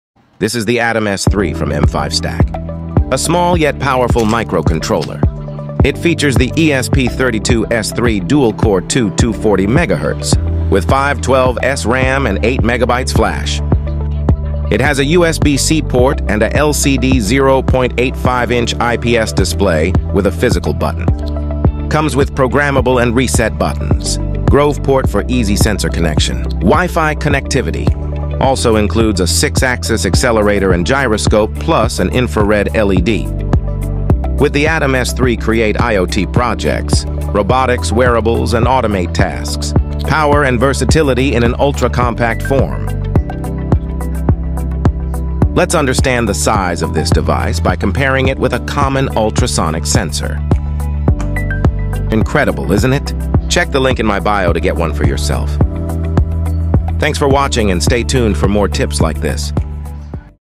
🇺🇸🇧🇷 🚨 Atom S3 from @M5Stack iot
🇺🇸🇧🇷 🚨 Discover the AtomS3: from @m5stack. A ESP32-S3 , dual-core 240 MHz, 512KB SRAM, 8MB Flash, 0.85" IPS display, Grove port, Wi-Fi, 6-axis sensor, IR LED, PWM, ADC, Duart, , and six pins for adding other devices. Perfect for IoT, robotics, and wearables!
🗣Follow me for more!👾🤖 @techtronic3d
Follow: @desbugando.se
Source: @techtronic3d